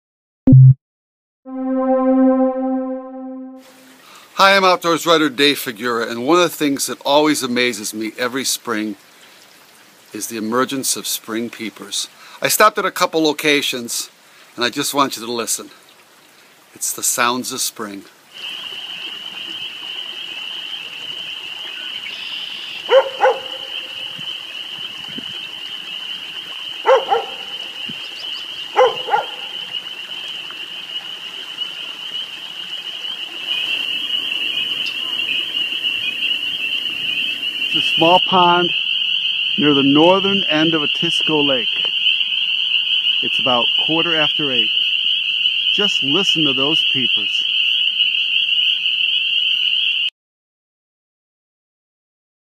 Listen to the sounds of spring peepers in Central NY
While trout fishing Sunday evening in the Skaneateles/Marcellus/Otisco area, outdoors writer David Figura couldn't help but notice the abundance and loud sounds of spring peepers.